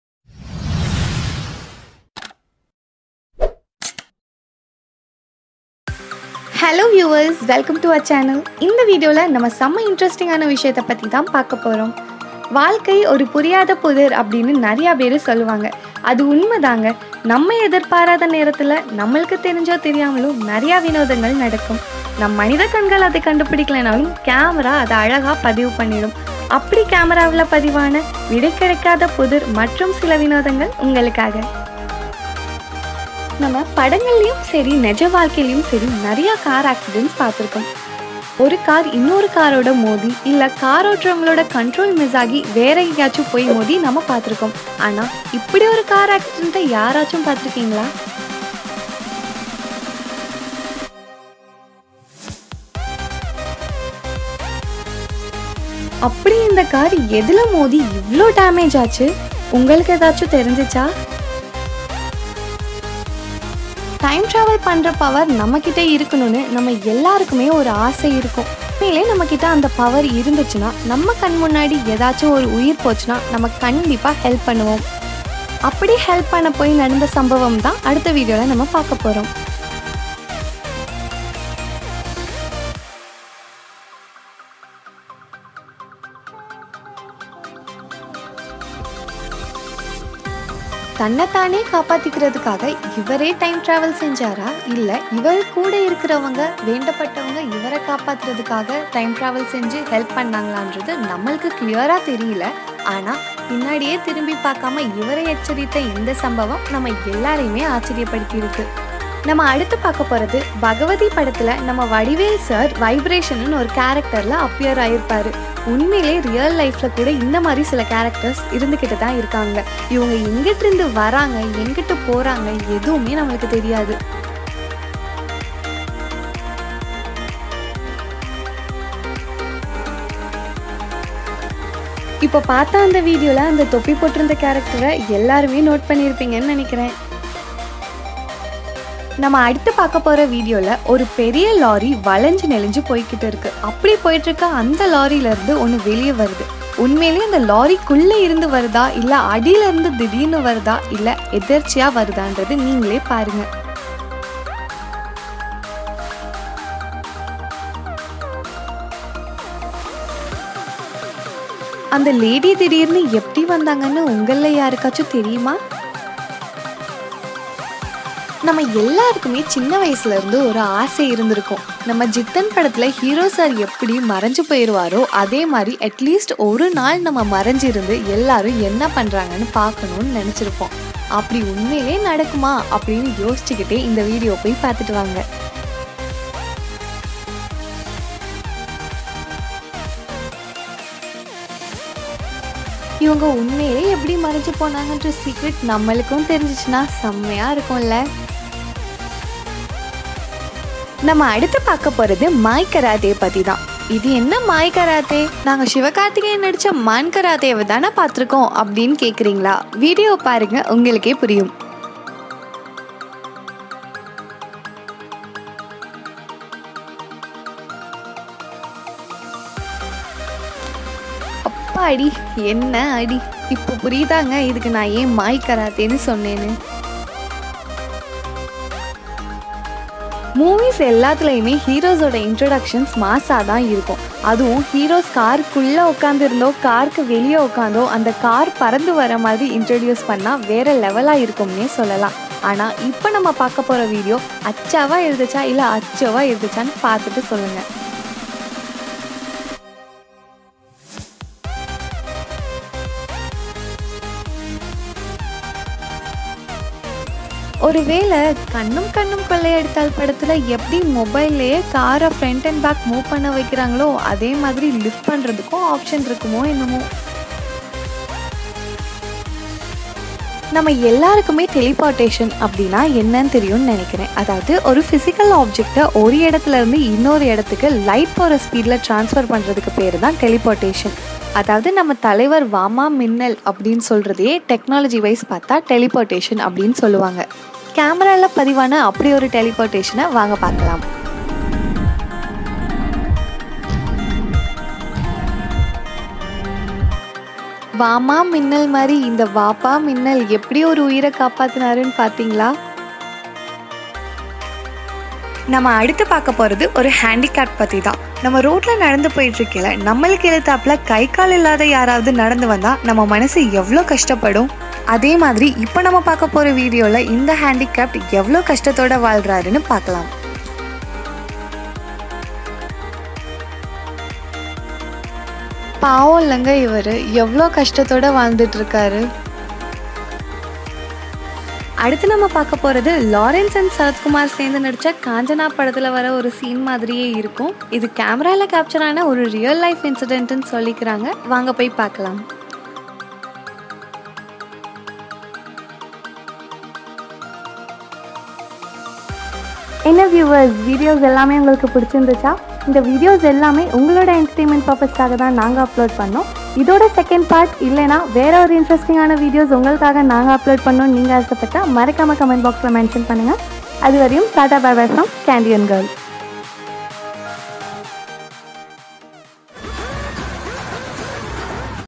10 weird and mysterious things caught on camera | பத்து வித்தியாசமான மர்மங்கள் |G Square
Have you ever seen a photo or a video which stops you in your tracks and make you wonder what's going on ?

There have been many videos which doesn't make sense and might show us somethings, although we are not entirely sure what...😳

There are many great mysteries that remain unexplained but have been caught on camera for us to wonder about.

Today we are showing you 10 unexplained mysteries caught on camera.